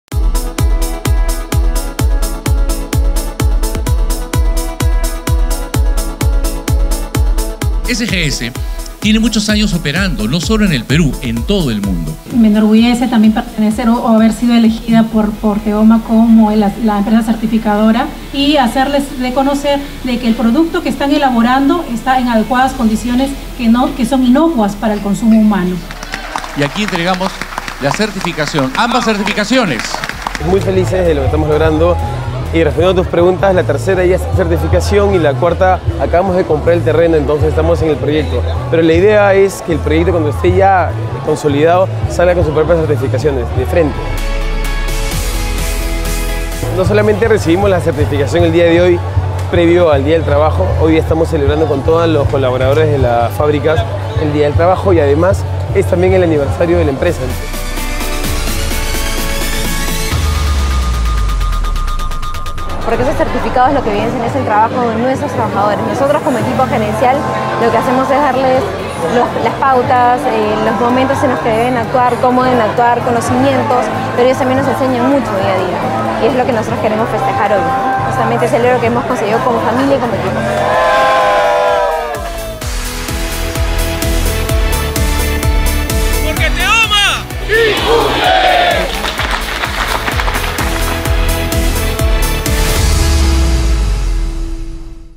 TEOMA PERÚ OBTUVO NUEVAMENTE LA DOBLE CERTIFICACIÓN INTERNACIONAL
La doble certificación de Teoma Perú es motivo de orgullo para todos sus colaboradores, ya que reconoce los productos que elabora esta empresa son los adecuados siendo inocuas para el consumo humano.

Felipe Novoa, Presidente Fundador, menciona que no solo celebran la certificación previo al día del trabajo, el cual también es festejado junto a sus colaboradores sino, también, es el aniversario de la empresa. Por lo que tienen varios motivos para estar contentos en ese día.

Además, Cynthia Tulliano, Gerente General Anditec Fusion, detalla que la certificación evidencia el trabajo de los colaboradores junto a la parte gerencial que dan las pautas, indican los momentos en los que deben actuar, como deben actuar y los conocimientos. Asimismo, ellos les enseñan algo nuevo día a día y quieren festejar el logro como familia y equipo.